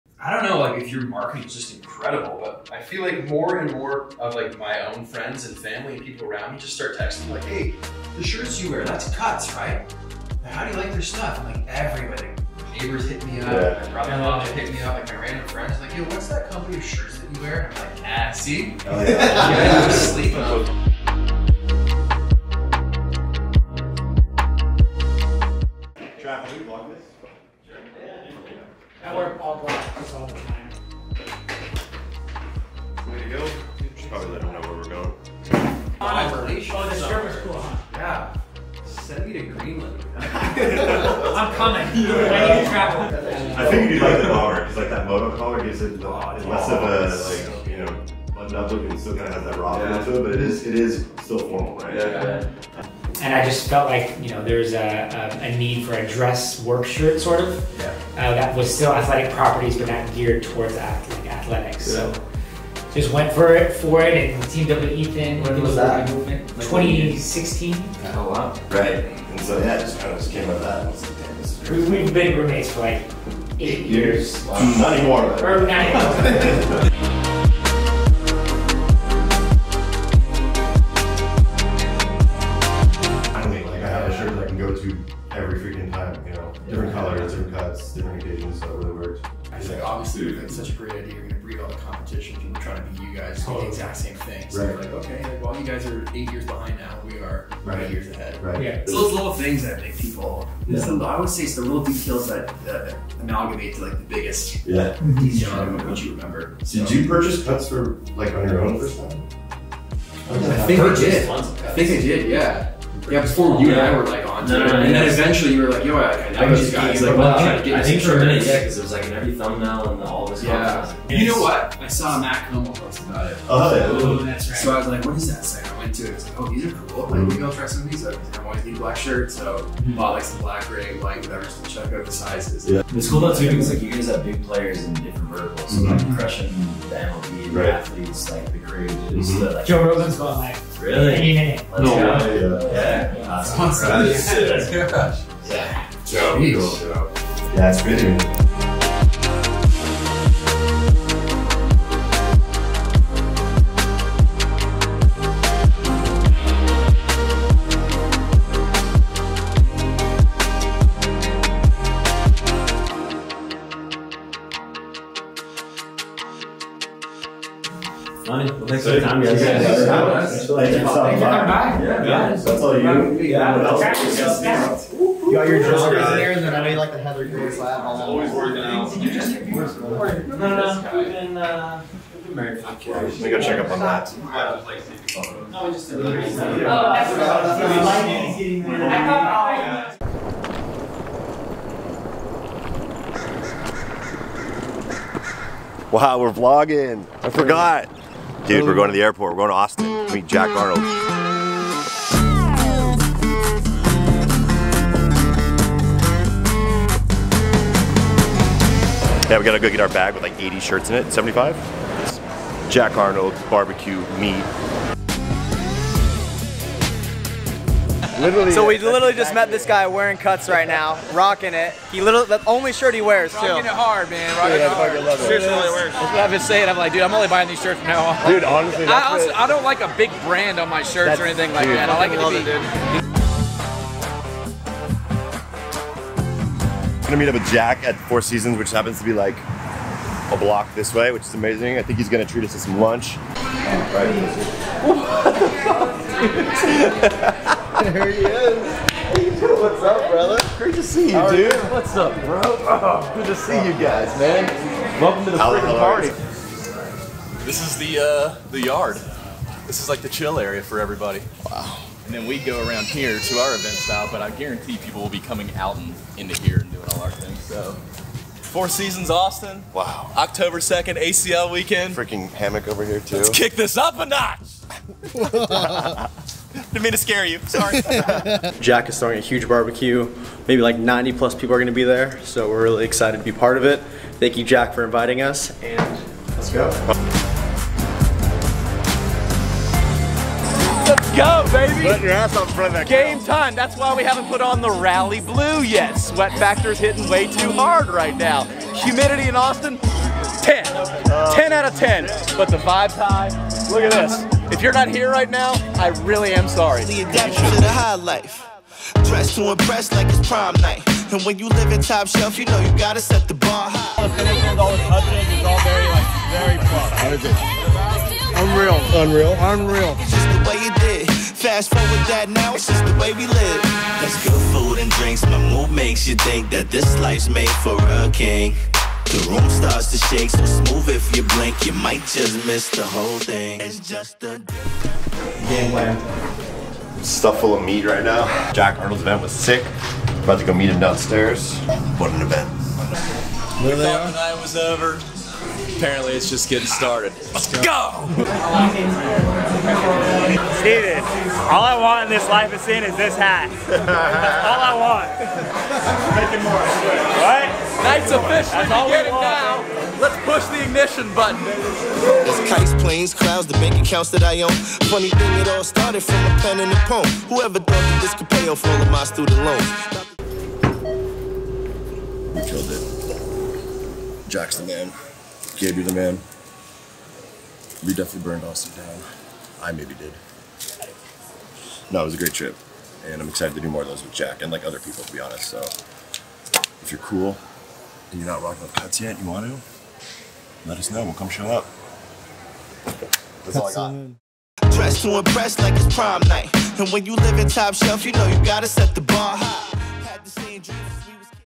For The Sport | Week 14 | Peter McKinnon Visits CUTS HQ + BBQ in Austin, TX
Last week we met up with some good friends of the brand. Peter McKinnon came to visit LA. Later in the week Ethan and Gabe flew to Austin, Texas for some quality BBQ with Jack Arnold.